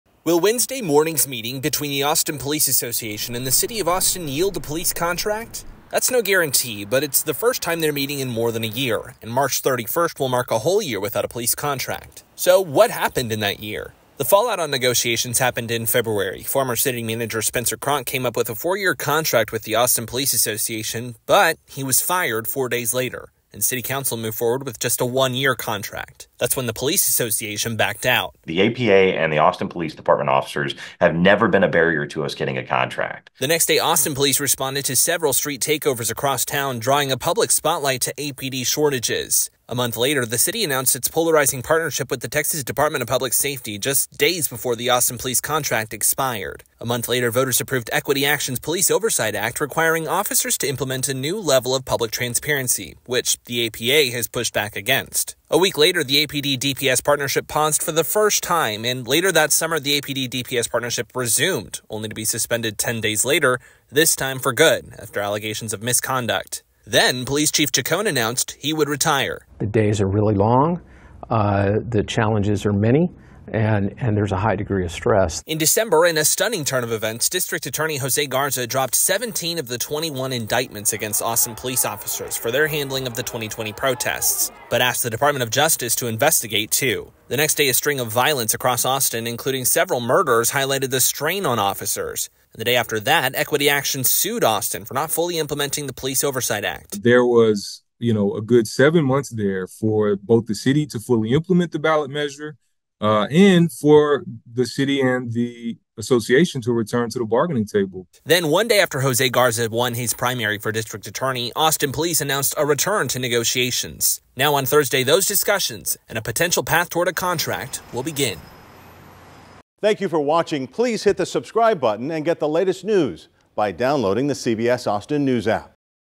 Police oversight at center of Austin contract talks
The Austin Police Association and the City of Austin will resume contract negotiations on March 13, 2024, after a year without a police contract, following a series of events including the firing of the city manager, voter approval of the Police Oversight Act, indictments and trials of officers, and a lawsuit against the city.